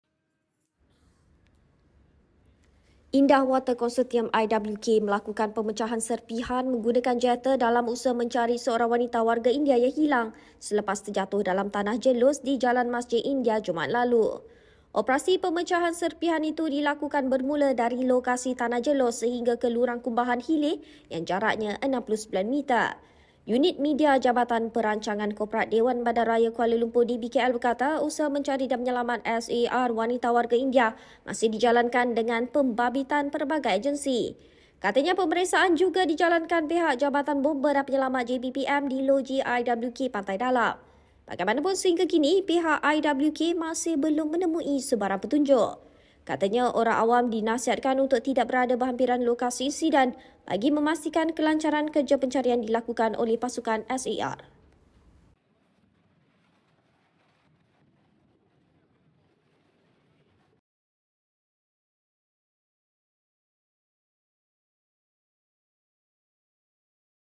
IWK guna 'jetter’ dalam operasi SAR mangsa tanah jerlus
Indah Water Konsortium (IWK) melakukan pemecahan serpihan menggunakan 'jetter' dalam usaha mencari seorang wanita warga India yang hilang selepas terjatuh dalam tanah jerlus di Jalan Masjid India Jumaat lalu.